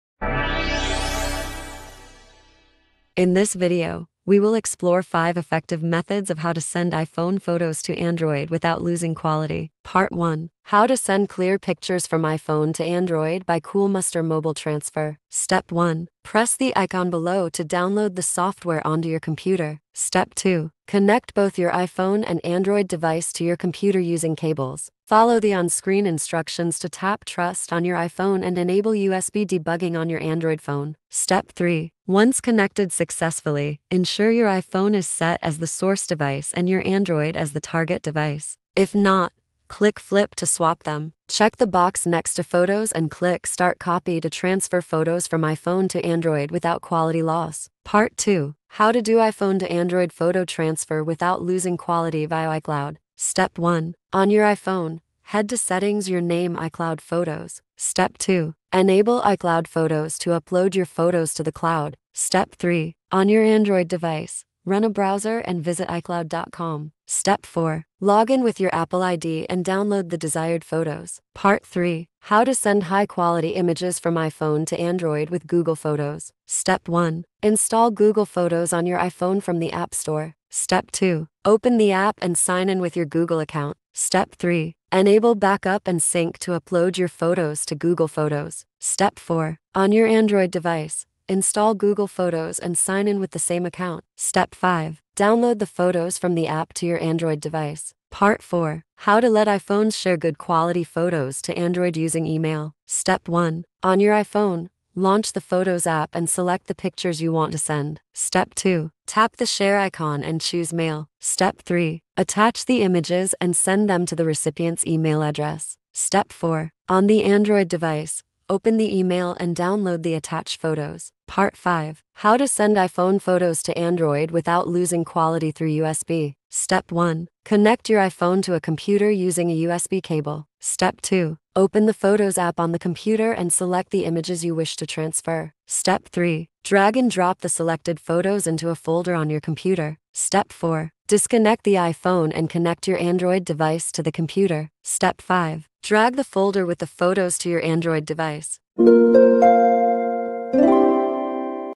How to Send iPhone Photos to Android Without Losing Quality? [5 Ways]
Learn how to send iPhone photos to Android without losing quality using Coolmuster Mobile Transfer, iCloud, Google Photos, and more.

00:00 Start
00:09 Part 1. How to Send Clear Pictures from iPhone to Android by Coolmuster Mobile Transfer
00:47 Part 2. How to Do iPhone to Android Photo Transfer Without Losing Quality via iCloud
01:11 Part 3. How to Send High Quality Images from iPhone to Android with Google Photos
01:37 Part 4. How to Let iPhones Share Good Quality Photos to Android Using Email
02:01 Part 5. How to Send iPhone Photos to Android Without Losing Quality Through USB